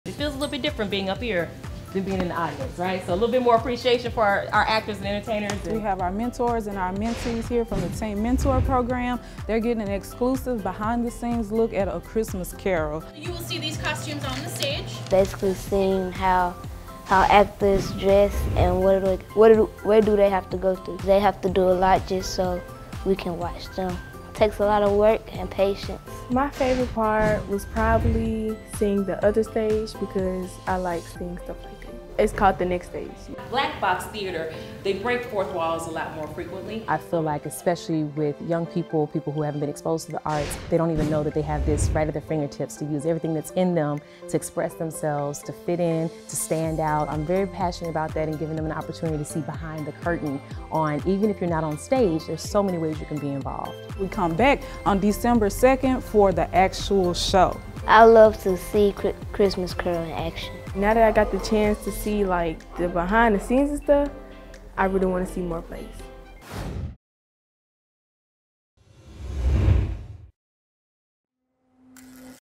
TEAM Mentor x Theatre Memphis Tour
The TEAM Mentor Program and Theatre Memphis partnered to bring a special backstage experience to program participants from all five school sites. Mentors and mentees had the chance to get a behind the scenes look at A Christmas Carol!

Memphis Grizzlies player Danny Green sponsored TEAM Mentor participants to enjoy the show and see how it all comes together!